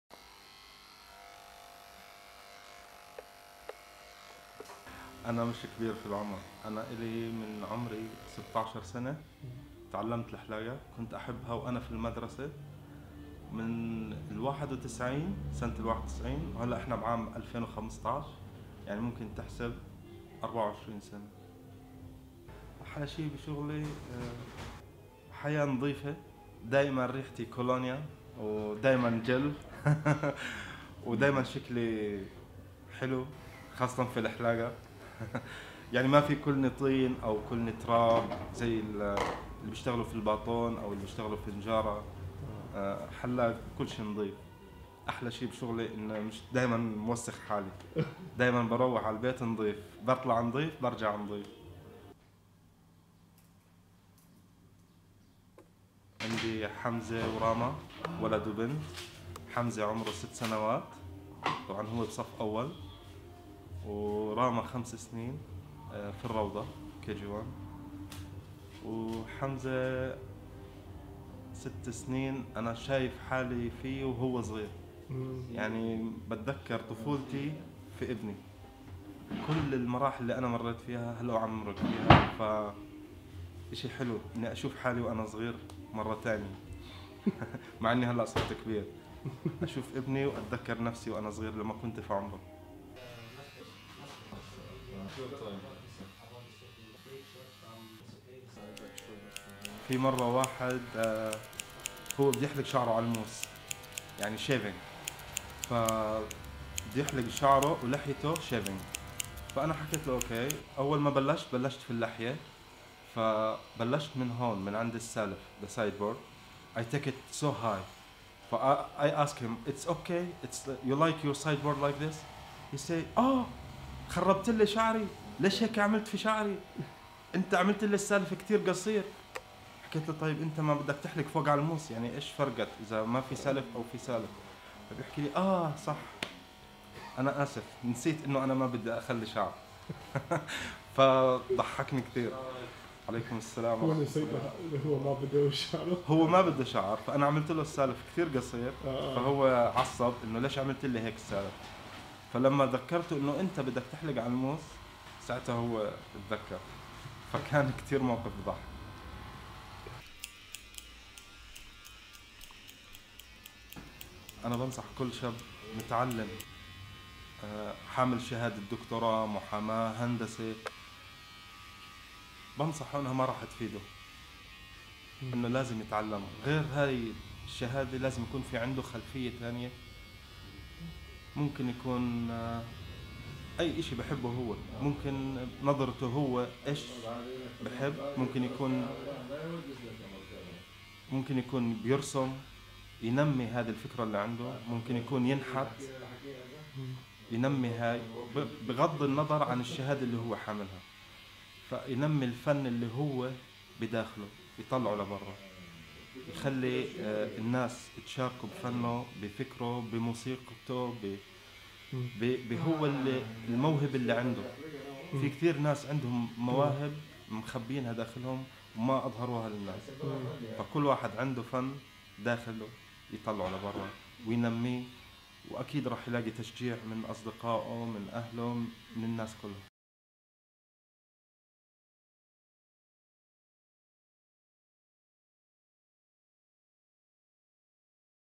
Hayatti::My Life - Mohammed
Hayatti or “My Life” is a new video series that introduces you to the experiences, life lessons learned, and narratives of the community members of Amman, Beirut, Jerusalem, and beyond. In Episode 4 we meet Mohammed, a barber from Amman, who talks about how he see's himself in his son and why he believes its important for people to foster, cultivate, and share their talents.